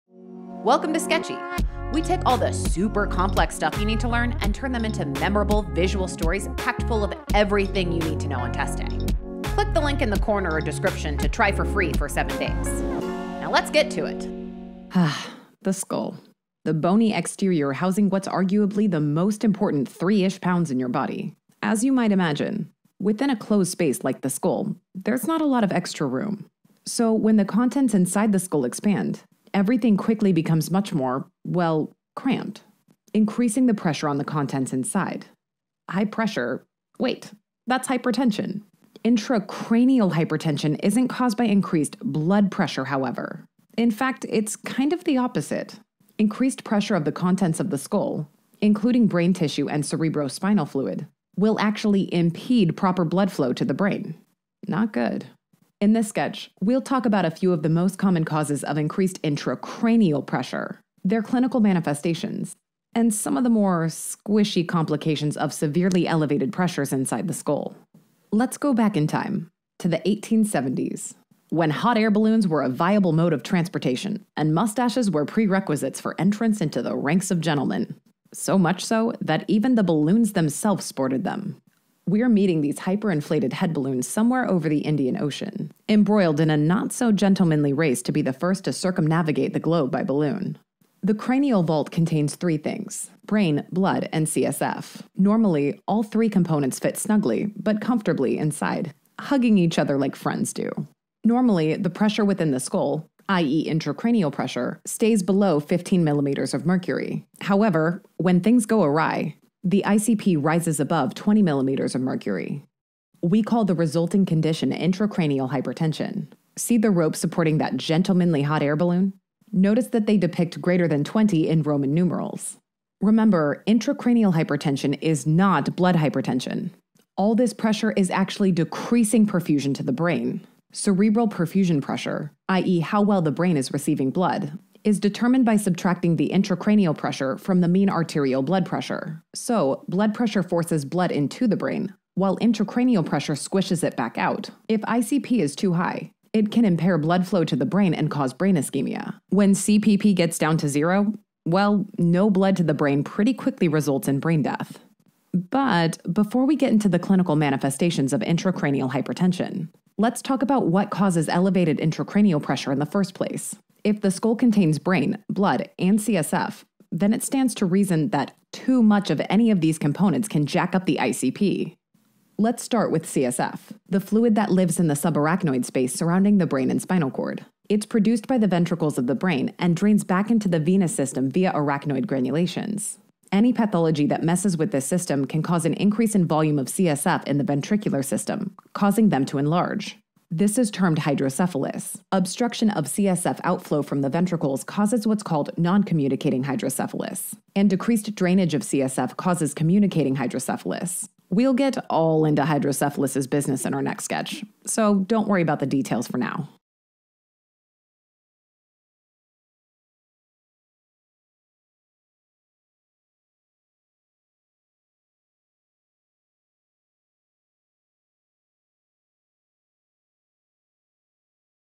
Intracranial Hypertension (Pathophysiology) | USMLE Step 1 | Sketchy Medical
We’re going up, up, and away into the skies for a thrilling ride on a bunch of hyperinflated head hot air balloons to talk about intracranial hypertension (ICH), or increased pressure inside the skull. We’ll explore causes of ICH like hydrocephalus and cerebral edema, and then delve into its clinical manifestations like altered mental status, vomiting, papilledema, and brain herniation. We’ll then set course for smoother waters and chat about management of ICH through treatment options such as hyperventilation, mannitol, and ventriculostomy. And we’ll conclude with a brief trip into Idiopathic Intracranial Hypertension (IIH), focusing on its risk factors, potential complications if left untreated, and treatment.